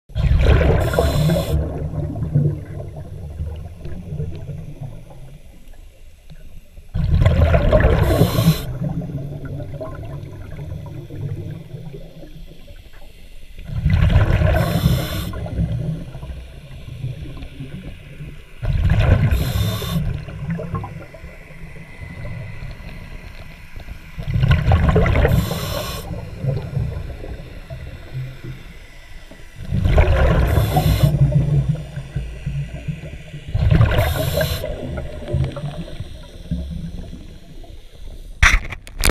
Bass protecting its babies
Largemouth bass. Niagara river